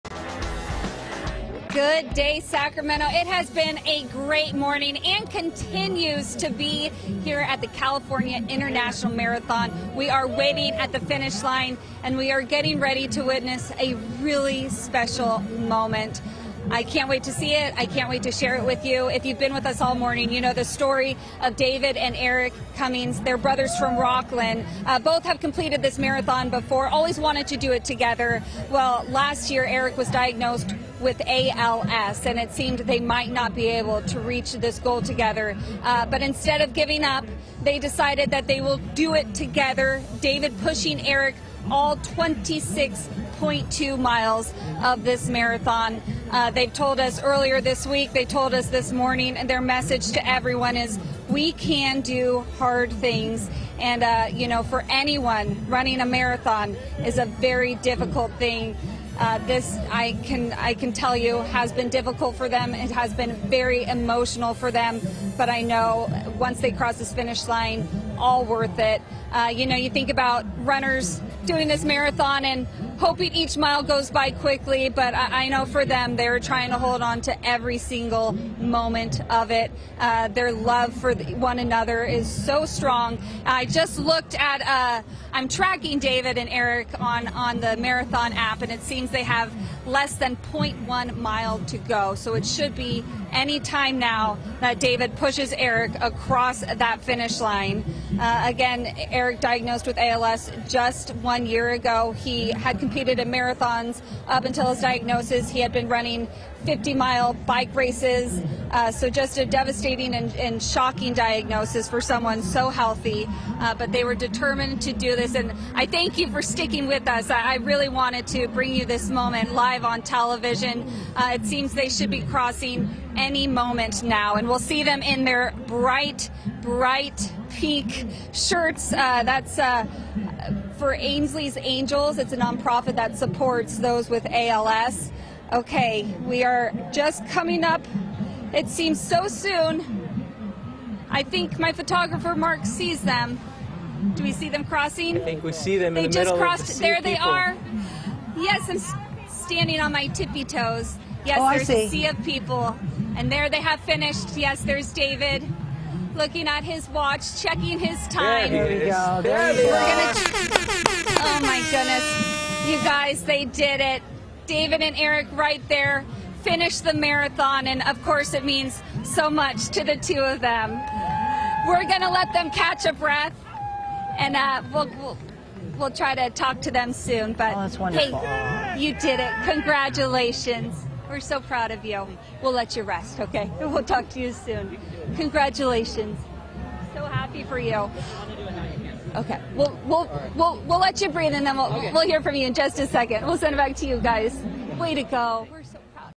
California International Marathon - The Cummings Brothers Cross the Finish Line!
Molly was there as the Cummings brothers finish the marathon!